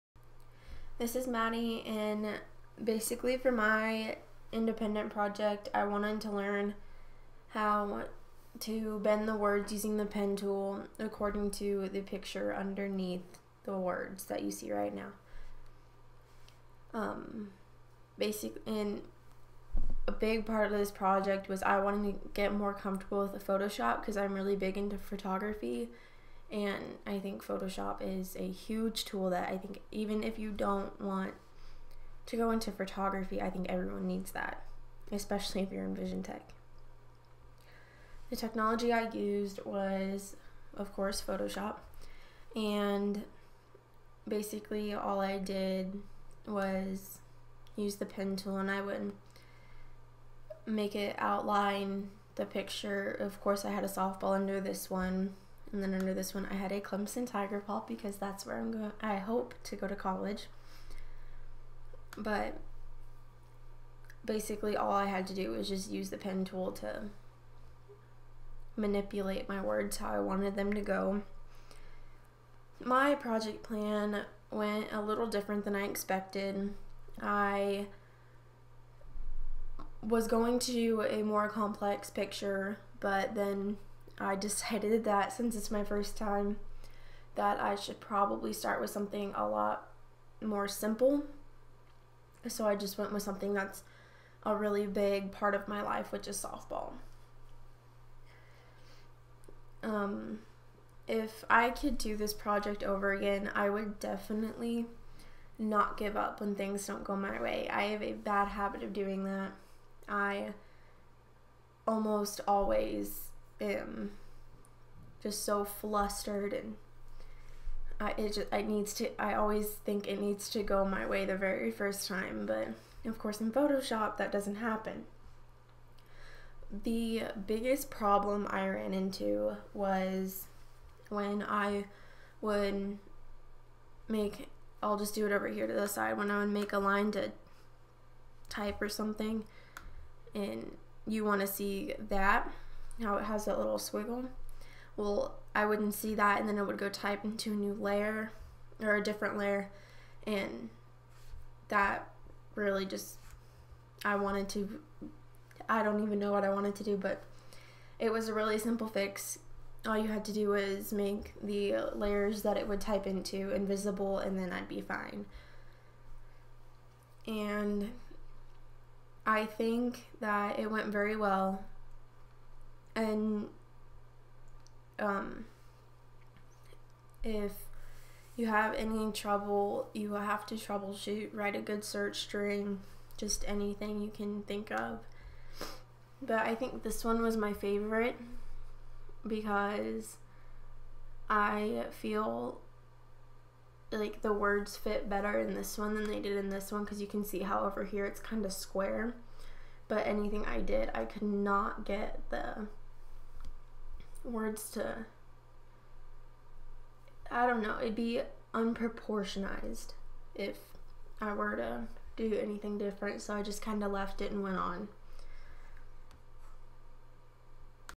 Typography with Photoshop CS6
My name is Madi and this is my typography project for Vision_Tek.